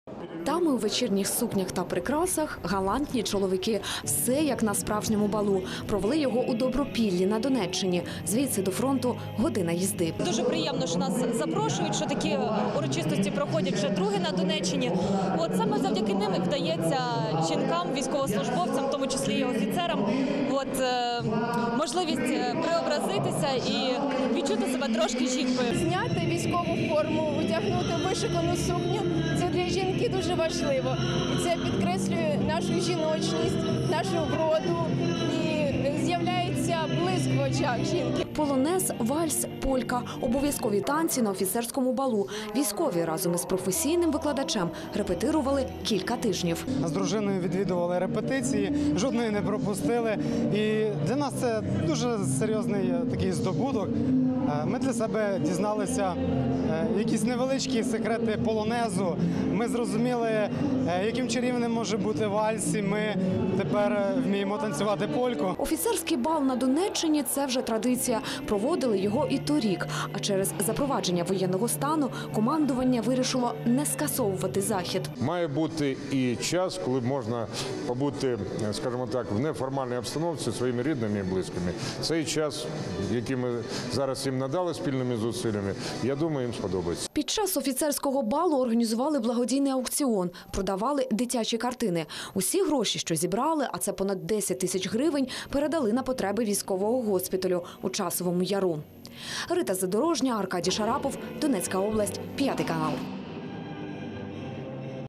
Вишукані сукні й посмішки на обличчях: на Донеччині пройшов офіцерський бал
Замість ворожих обстрілів, чути музику. Офіцерський бал організували у Добропіллі на Донеччині. Серед учасників - військовослужбовці Операції об’єднаних сил. До балу готувалися кілька тижнів - проводили репетиції з професійним танцівником.